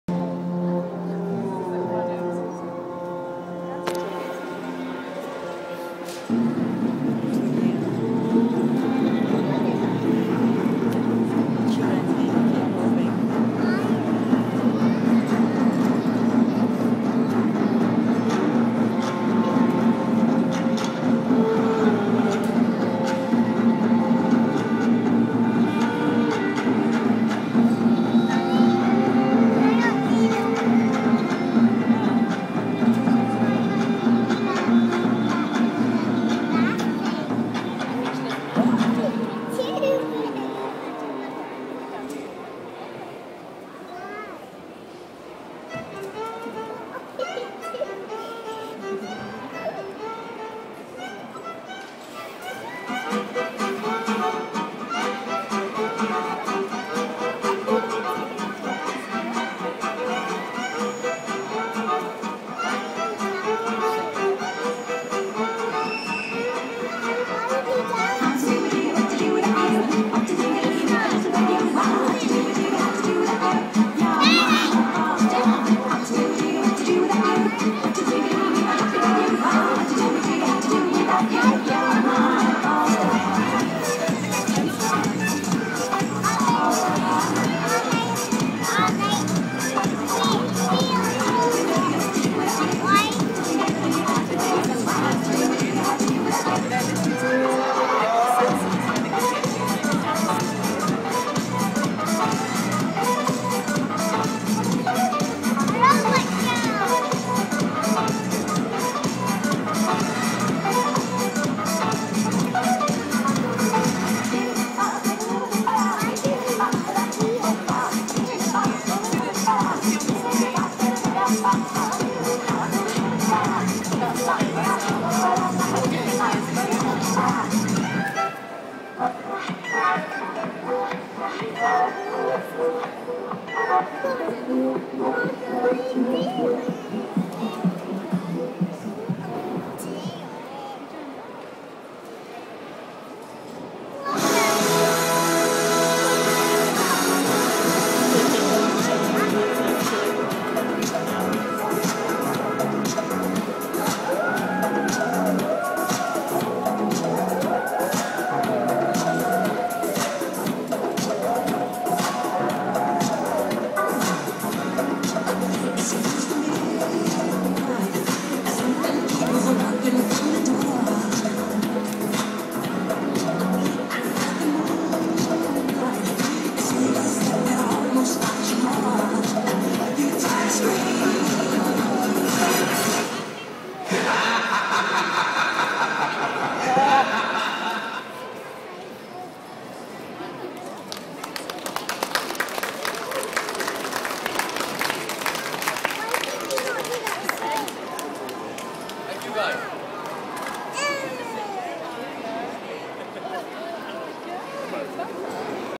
Dancing Robots - Edinburgh Science Festival 2012
Dancing robots in the National Museum of Scotland
Part of the Edinburgh International Science Festival 2012

Comment and share please..